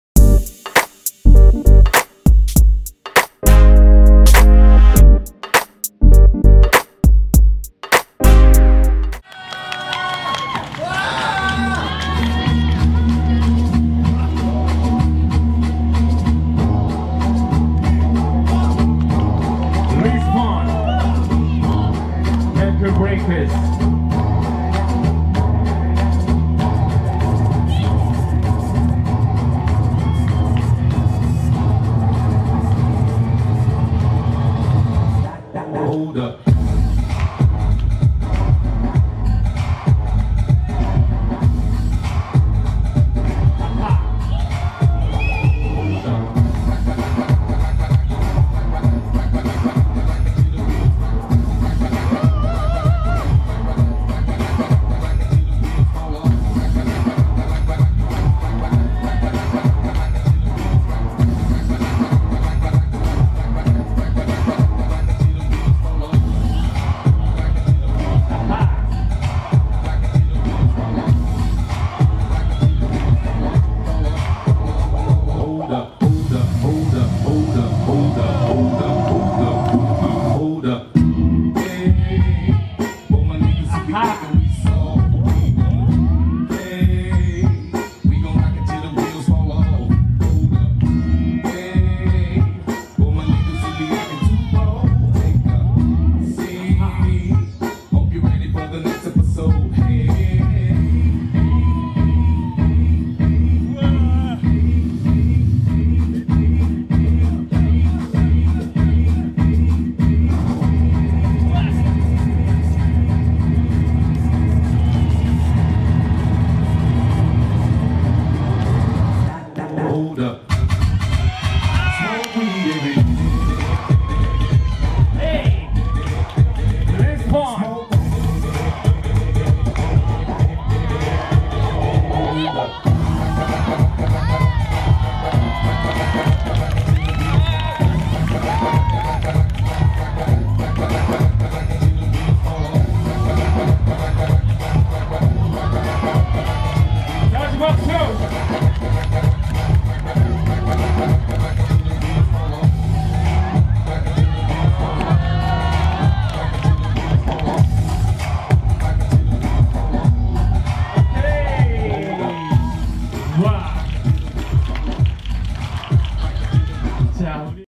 RESPON @2018 RESPECT FESTIVAL - KEEP SMILE
2018 RESPECT FESTIVAL - KEEP SMILE

제38회 장애인의 날 기념 야외 축하행사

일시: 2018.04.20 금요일

장소: 이룸센터프라자

주관: 장애인의 날 행사준비위원회

후원: 보건복지부, 이룸센터

HOST: MC GO / N.Kyung Hee

DJ: MARCIA(RunDJ CREW)

SHOWCASE
BIBBLY GUMPS
O.O.S
ANIMATION CREW
ANGELKIT BAND
HORIM BAND
KOREA BEST DANCERS JAM SESSION